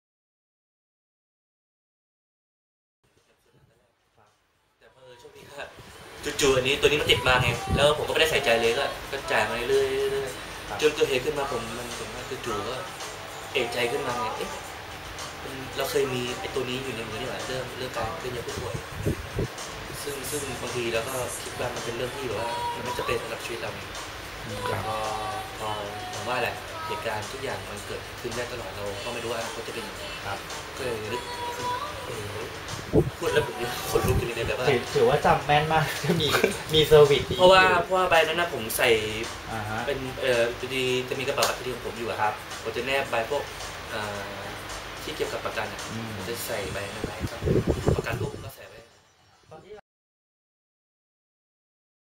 Assist America Testimonial - Thailand
"...then the accident happened. I then recalled I have medical assistance."